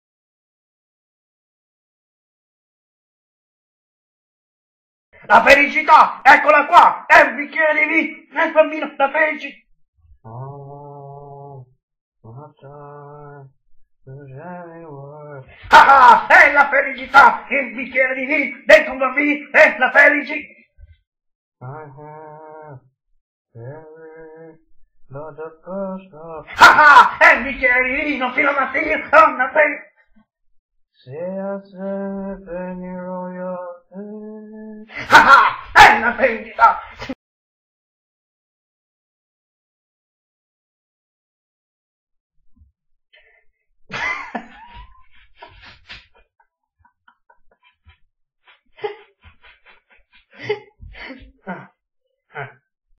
Sbalzi Di Umore Di Kurt Cobain
Anche su Youtube uno storico rperto del grande rocker prematuramente e tragicamente scomparso nel 1994.